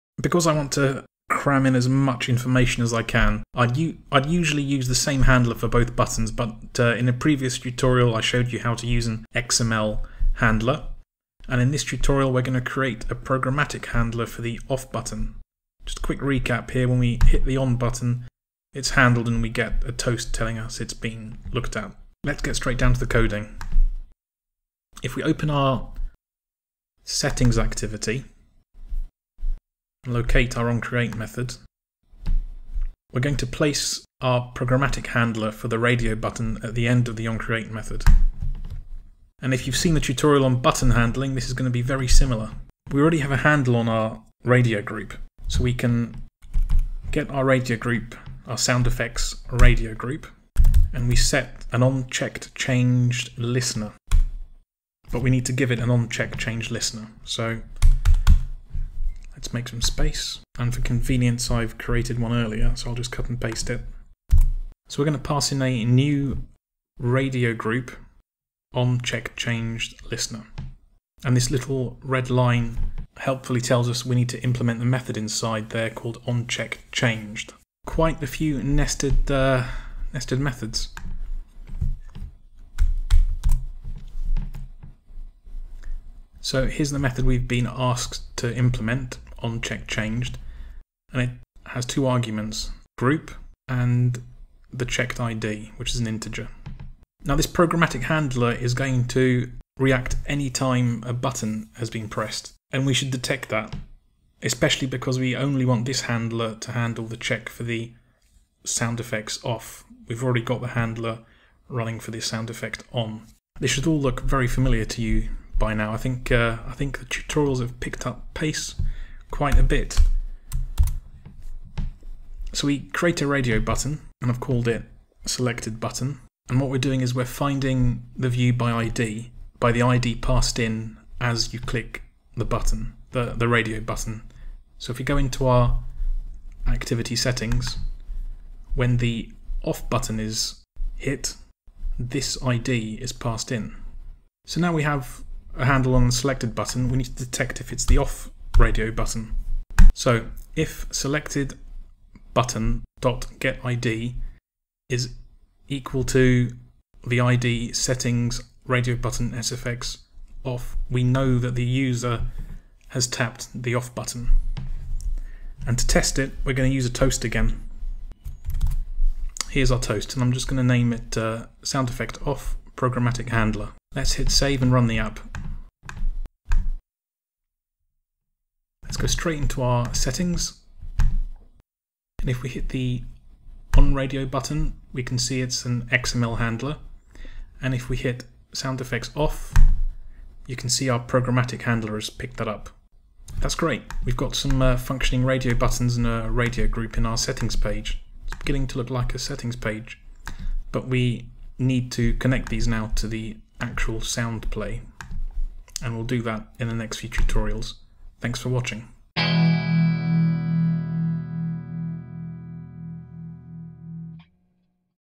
Android Tutorial #29: Settings Page (3/7) Detect and handle radio button programmatically
I show you how to handle a radio button event programmatically using, setOncheckedChangeListener and onCheckedChanged.

Usually I would use the same handler for the same button group, but you can use different handlers and I do that here to cram as much info in to the tutorial.